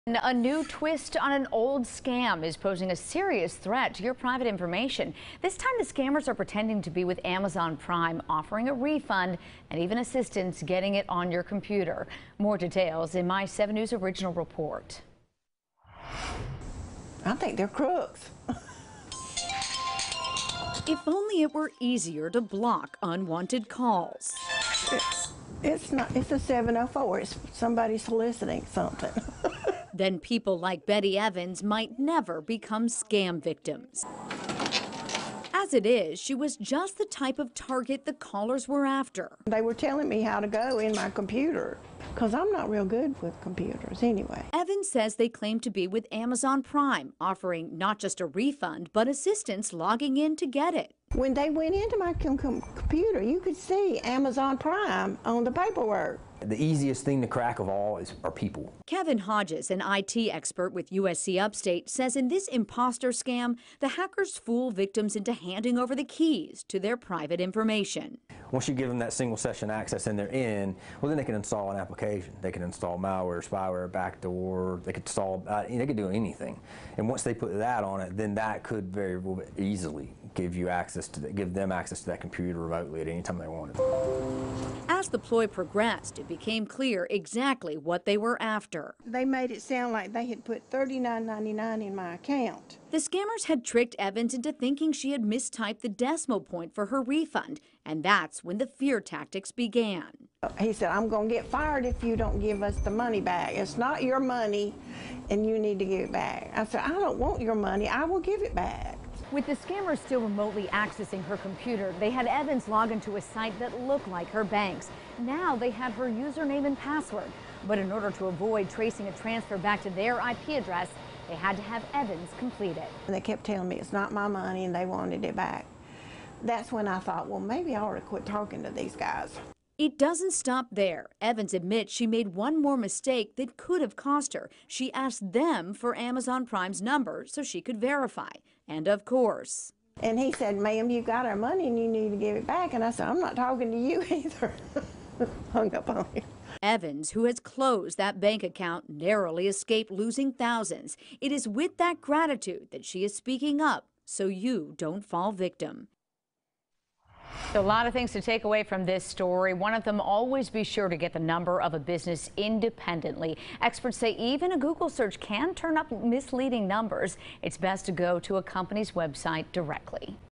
Beware: Amazon Prime scam could cost you
A new twist on an old scam is posing a serious threat to your private information.